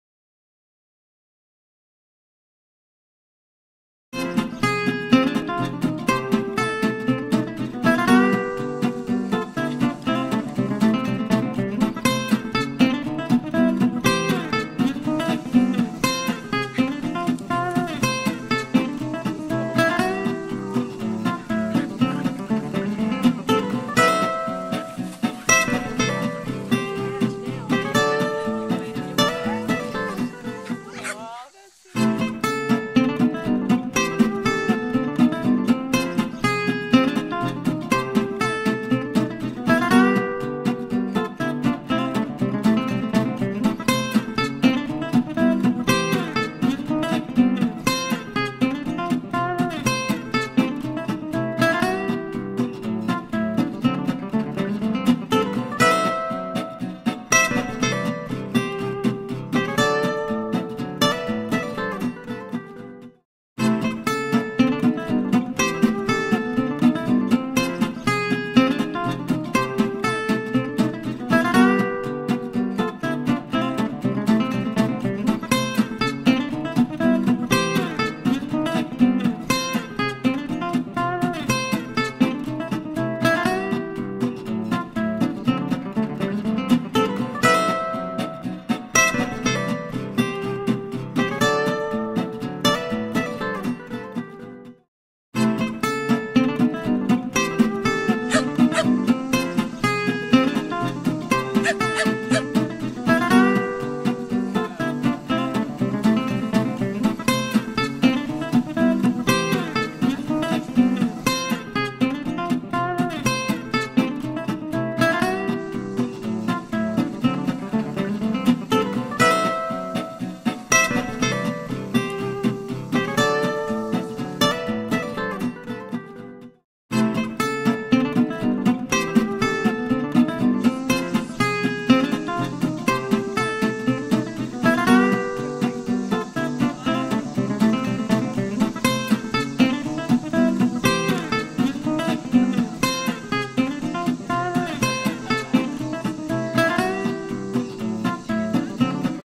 The Many Ways Dogs Initiate Play
Just about everyone knows and can recognize a play bow, but that's not the only signal dogs use to solicit play. See video examples of behaviors dogs use to initiate social play with another dog. While watching each segment note these behaviors can occur together in random succession or alone as isolated behaviors.